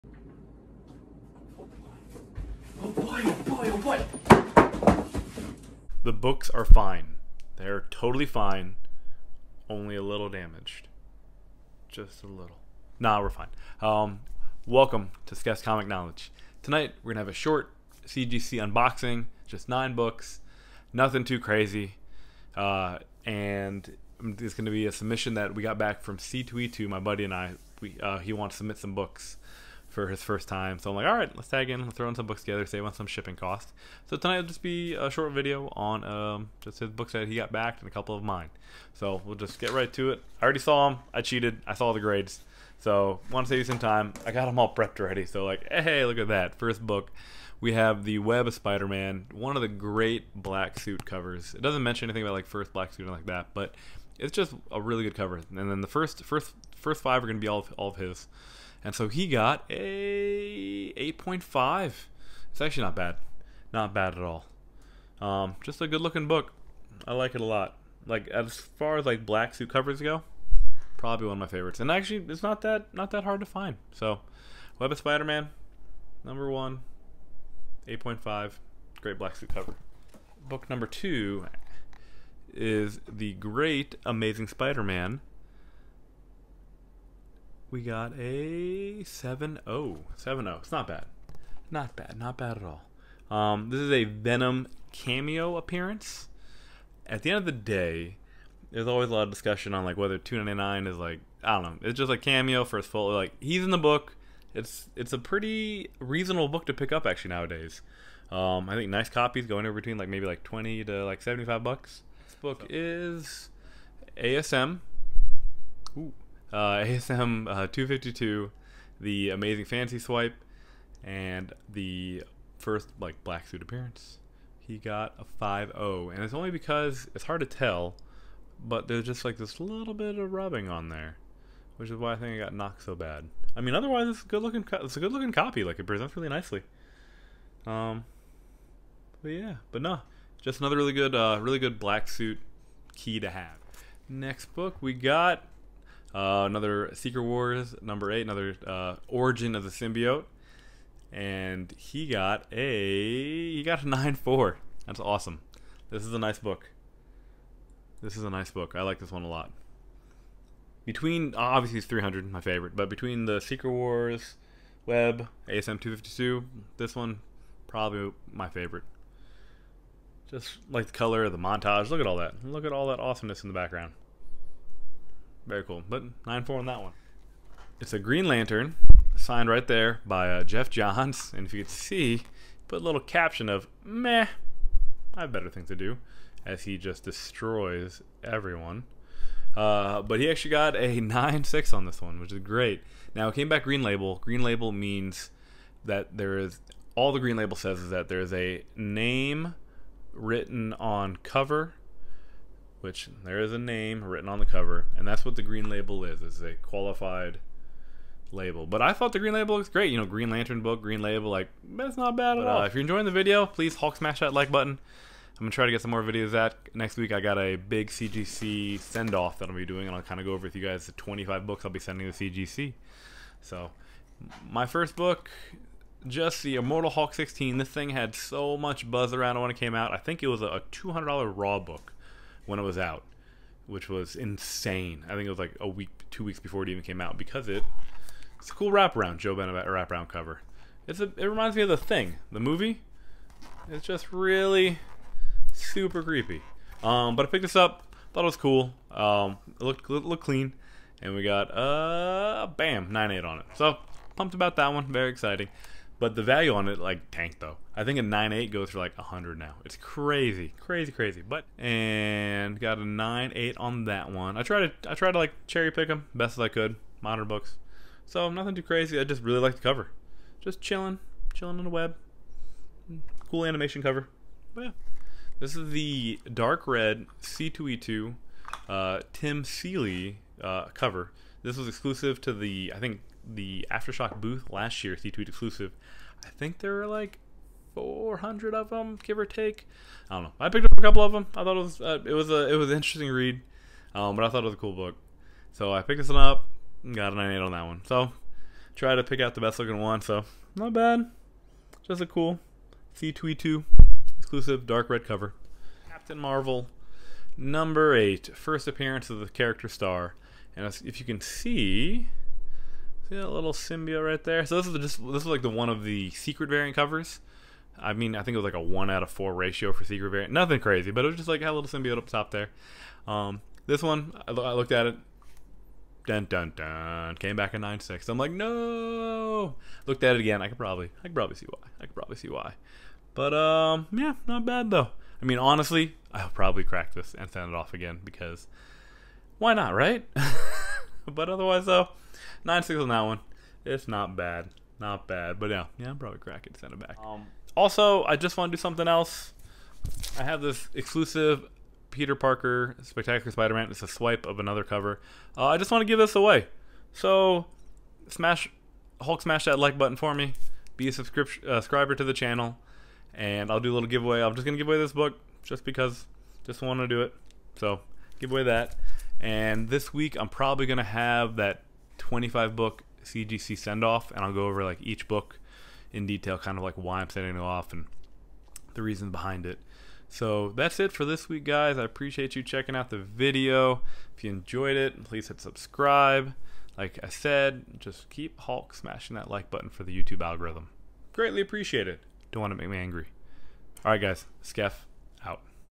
Cgc unboxing !
Hey comic community!

Thanks for stopping by skeffs comic knowledge!

Today we have a cgc unboxing of a couple ASM keys and some new moderns that my friend and I dropped of at c2e2